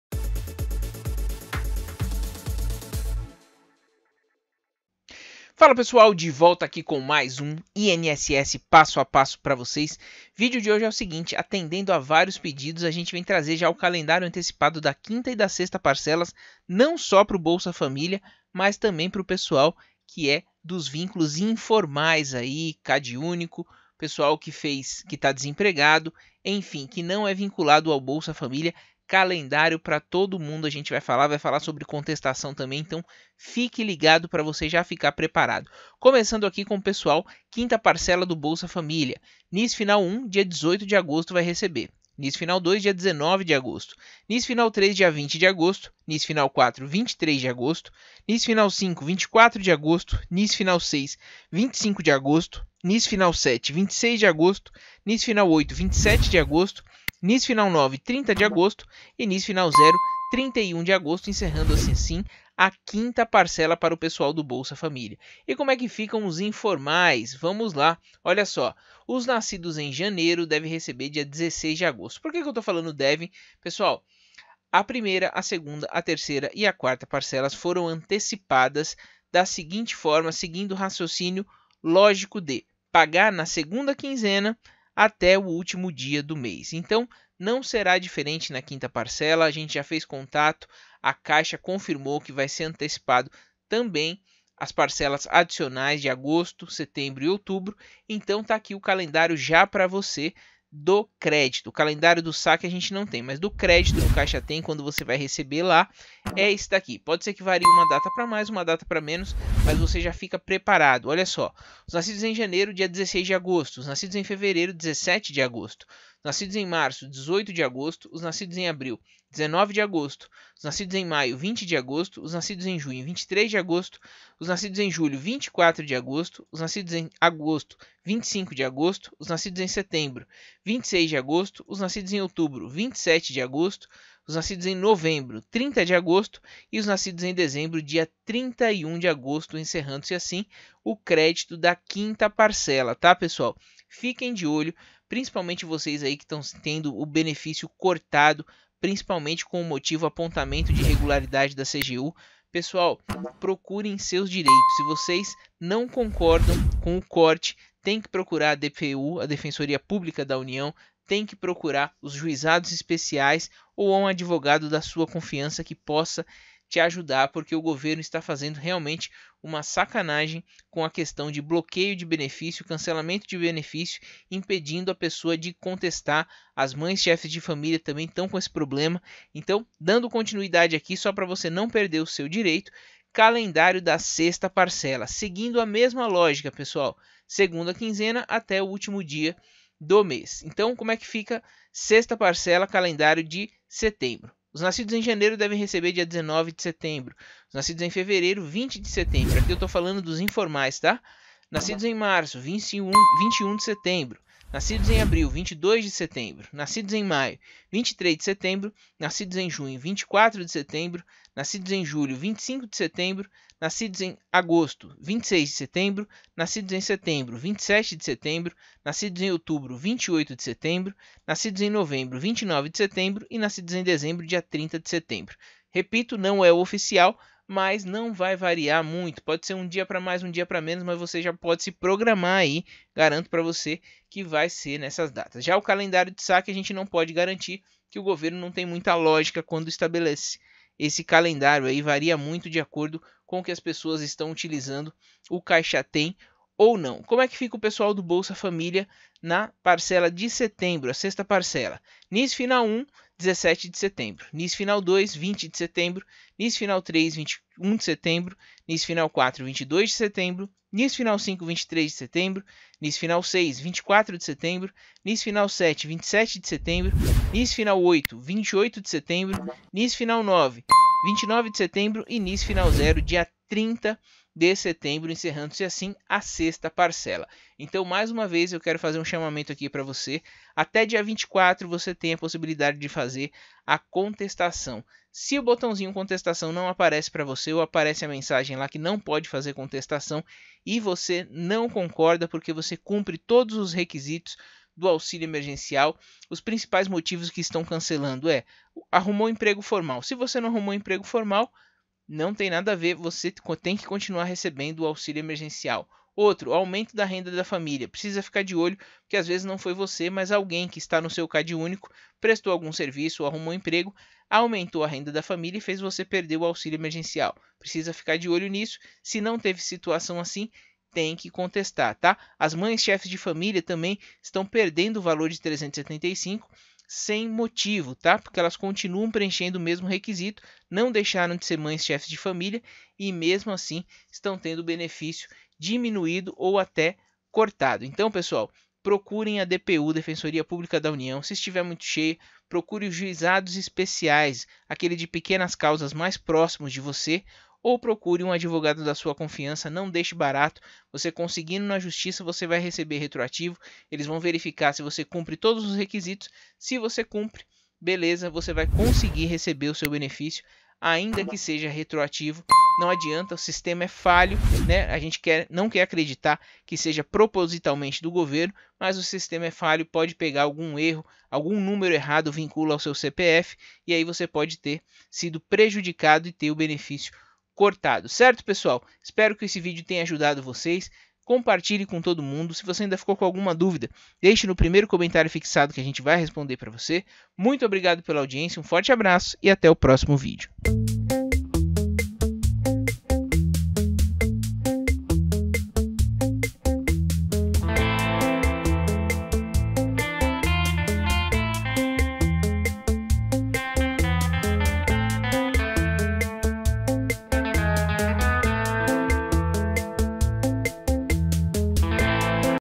CALENDÁRIO da 5º e 6º PARCELAS DO AUXÍLIO EMERGENCIAL ANTECIPADO PARA INFORMAIS E BOLSA FAMÍLIA
Calendário da 5º parcela do auxílio emergencial / Calendário da 6º parcela do auxílio emergencial. Afinal, quais serão as datas de pagamento da quinta e da sexta parcela do auxílio emergencial?

Saiba tudo neste vídeo sobre o calendário antecipado da 5º e da 6º parcela do auxílio emergencial 2021.

░T░i░k░t░o░k░:░ ░G░a░n░h░e░ ░a░t░é░ ░R░$░8░5░,░0░0░ ░u░s░a░n░d░o░ ░n░o░s░s░o░ ░c░ó░d░i░g░o░ ░d░e░ ░c░o░n░v░i░t░e░J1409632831